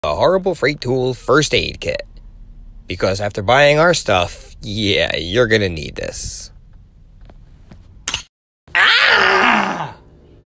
Horrible freight tool- first aid kit
Another Horrible Freight Tool ad

A first aid kit

A parody on Harbor Freight Tools